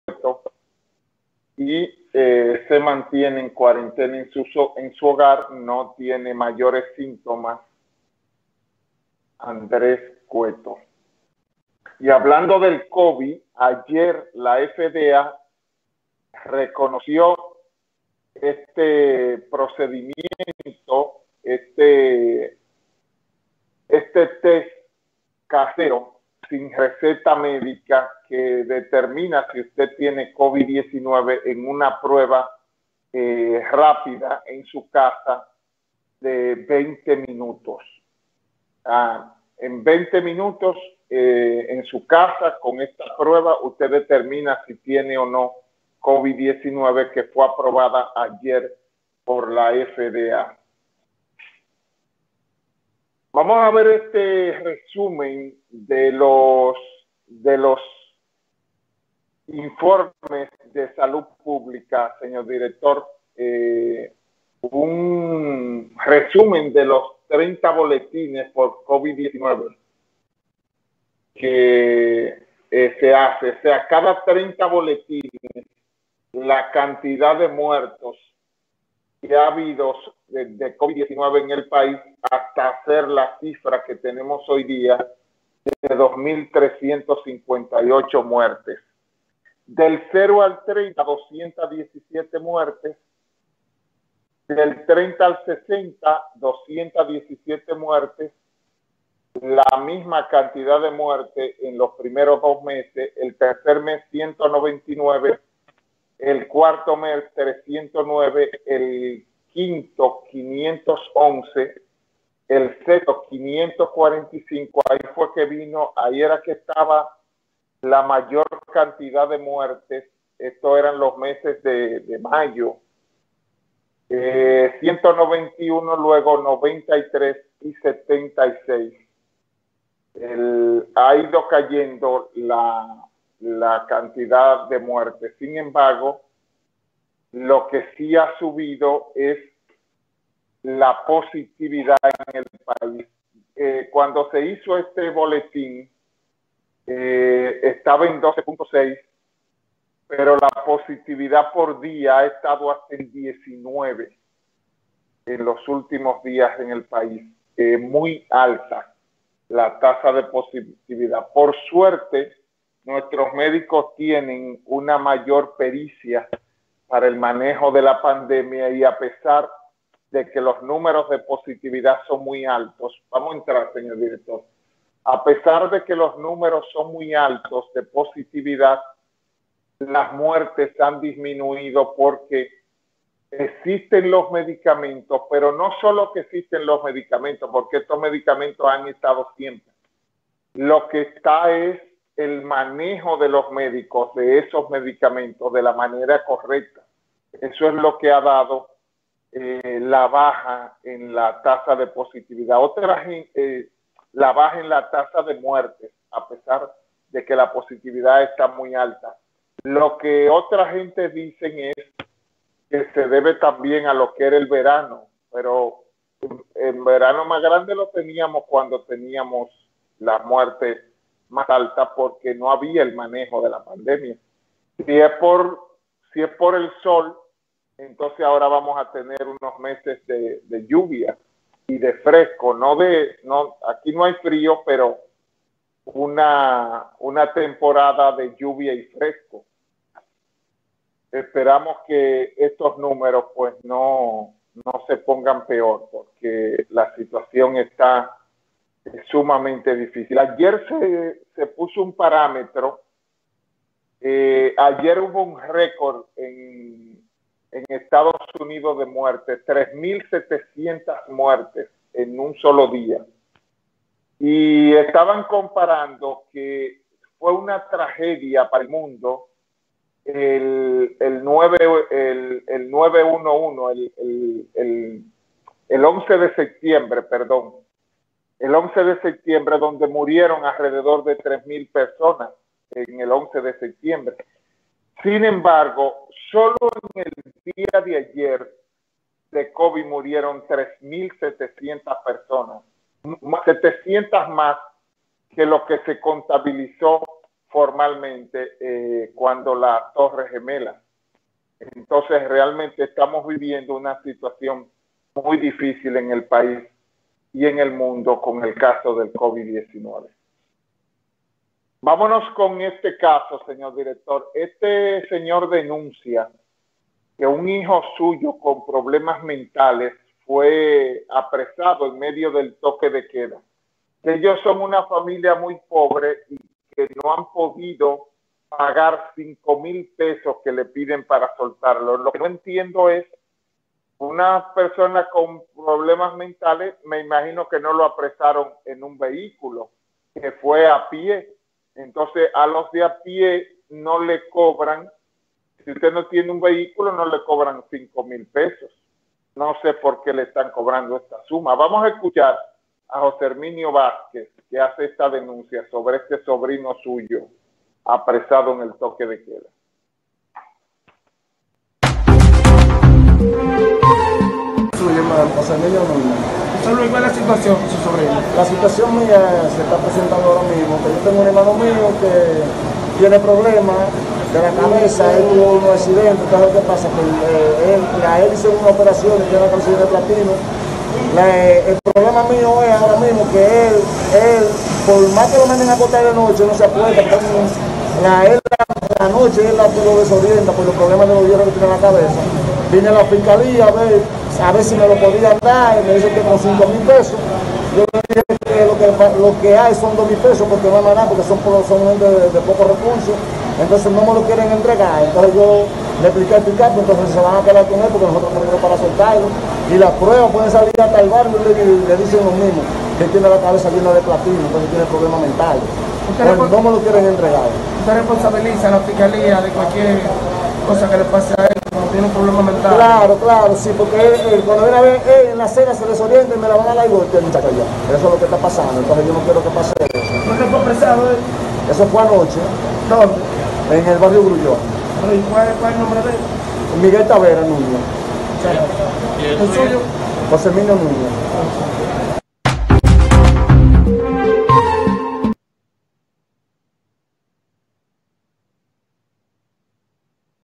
Hombre denuncia PN apresó a familiar que padece problemas mentales durante toque de queda
Hombre denuncia PN apresó a familiar que padece problemas mentales durante toque de queda Lo cuenta José Manuel Frías